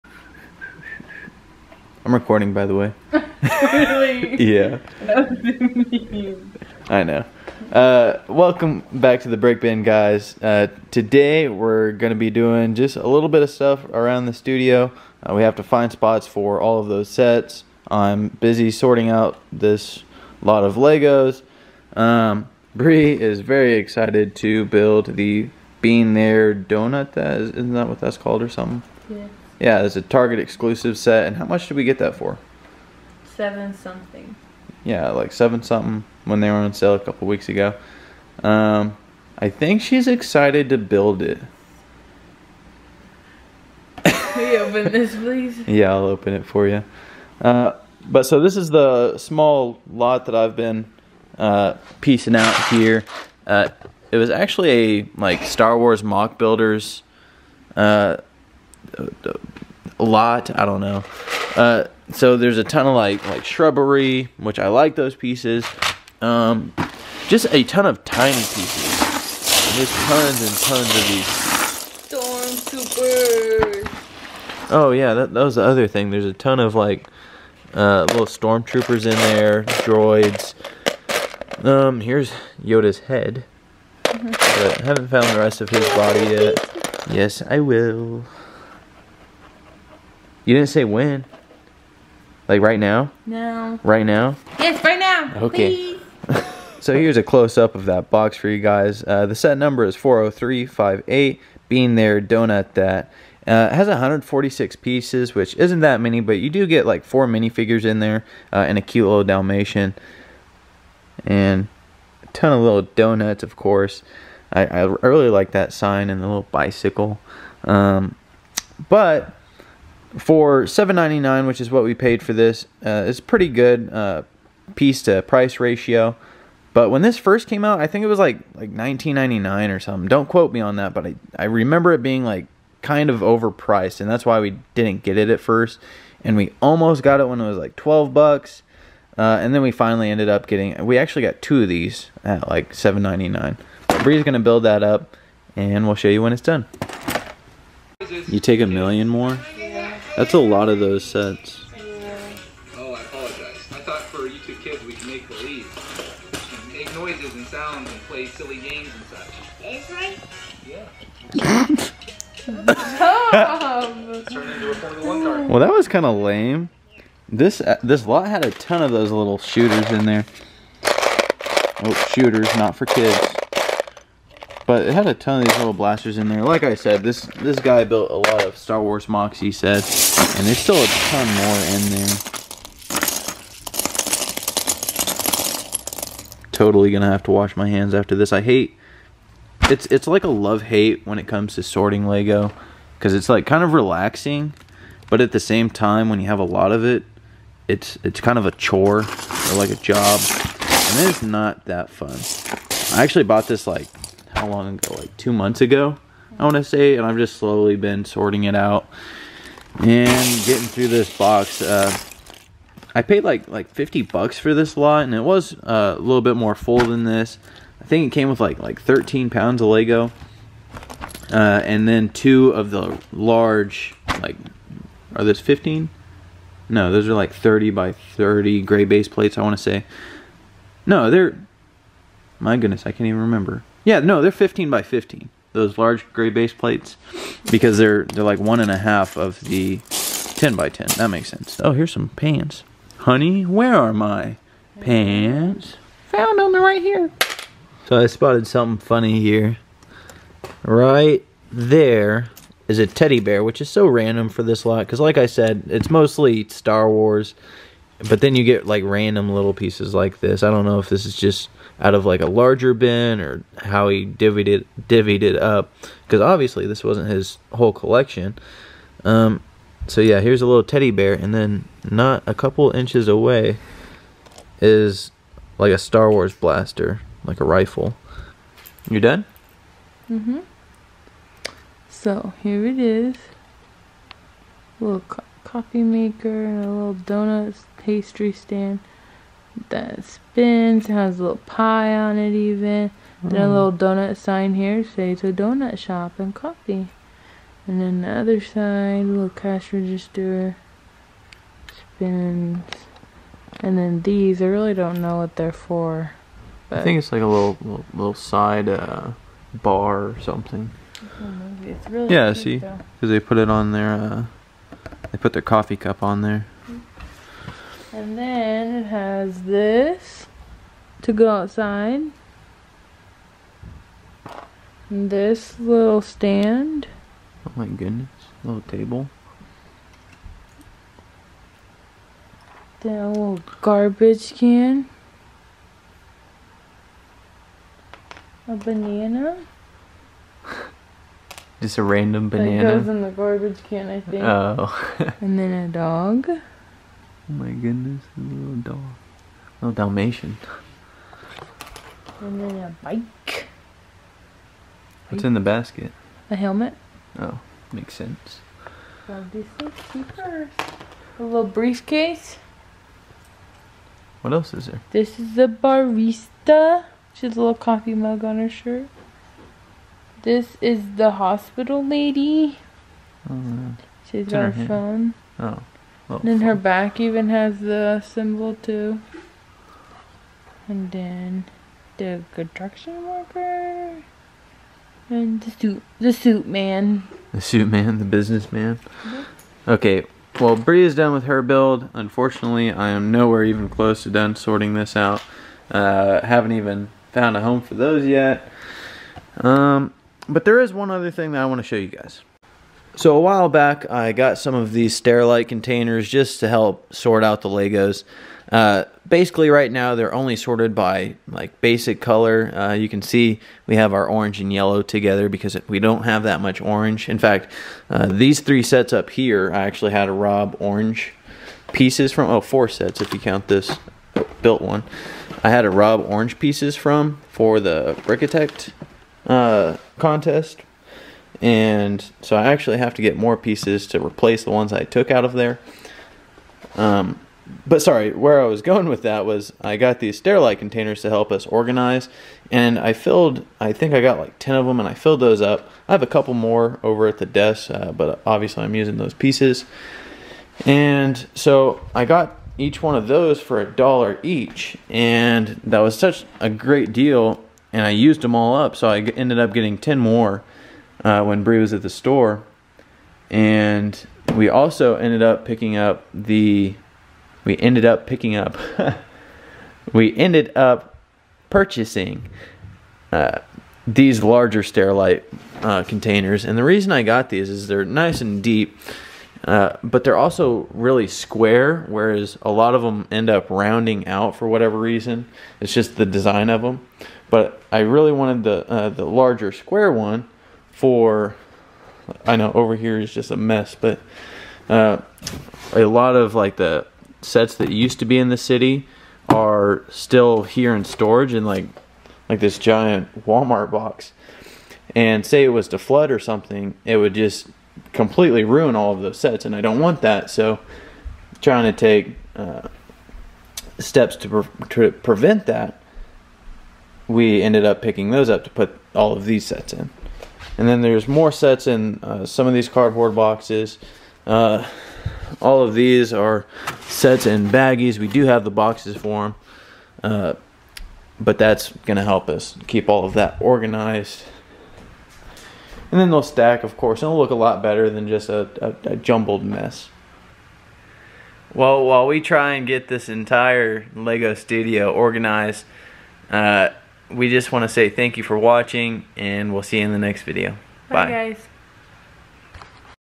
Shooting GF with Lego Stud Shooter! Build & Sorting Haul
Join B&D as we Build, Sort, and organize!
Lego Bean There Donut That (40358)